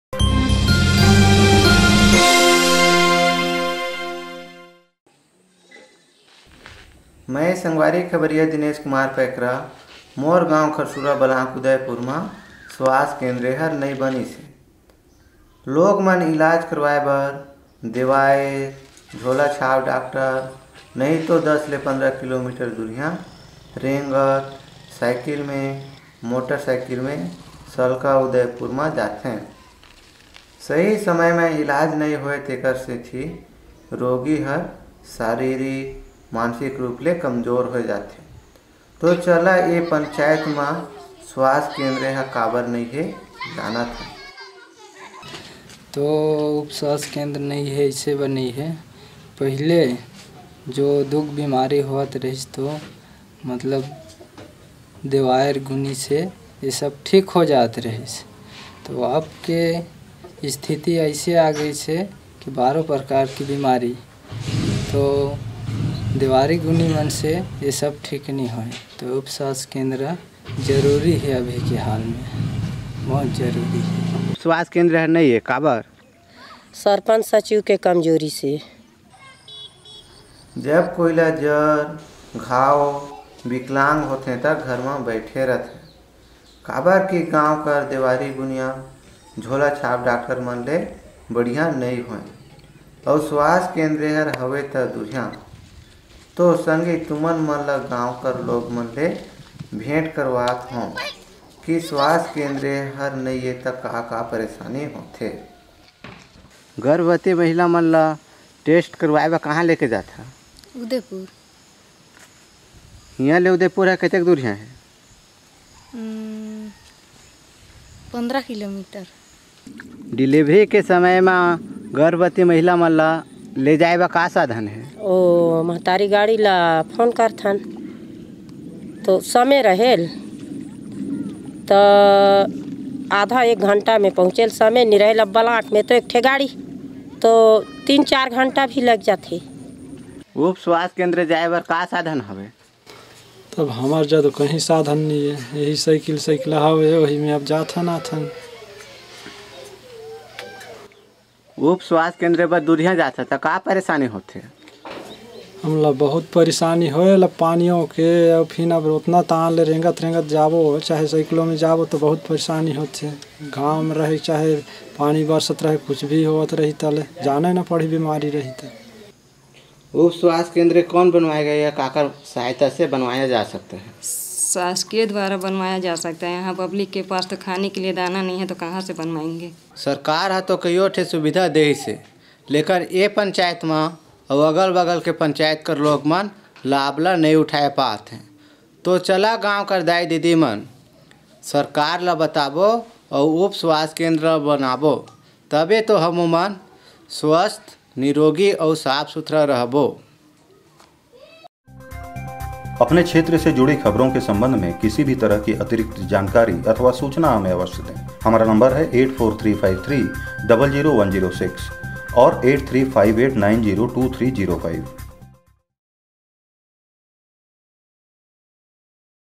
Participatory video: Waiting for health facility
Report By: Dinesh Kumar Paikra

Synopsis: Villagers of Kharsura Gram Pnachayat (Chhattisgarh) are waiting for primary health center (PHC) since the inception of this village. Due to this gap, villagers are forced to visit private and fake doctors in their village. Nearest PHC is 15 km away from the village. Lack of health system is affecting all the age groups and also fostering superstitious believes.